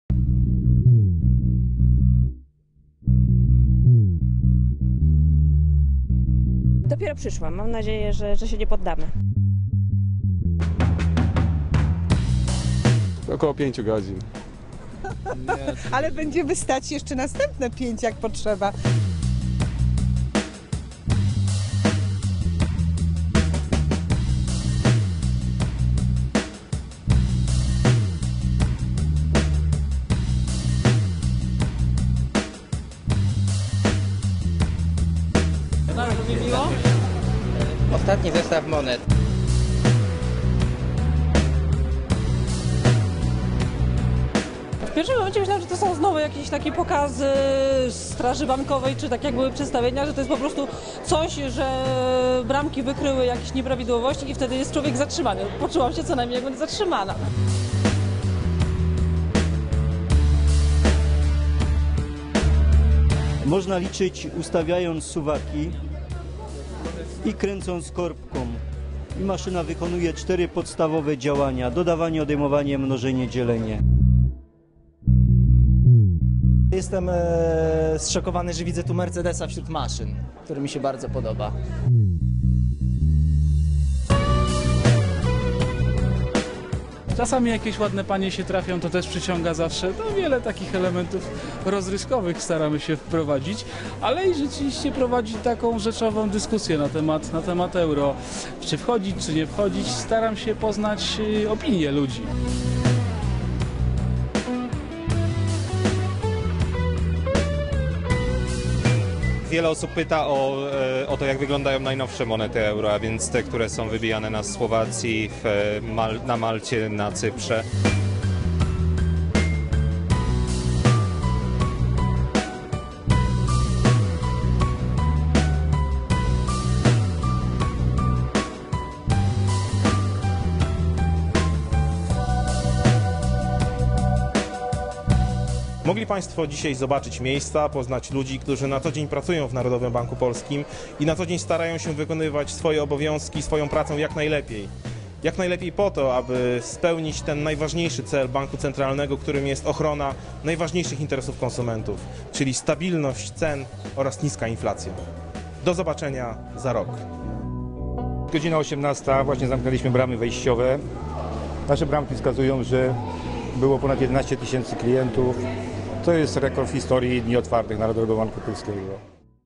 Dni otwarte NBP '2009 (cz. 7/7)
Siódma i ostatnia część reportażu z dni otwartych w centrali Narodowego Banku Polskiego w Warszawie, które odbyły się 6 i 7 czerwca 2009 r.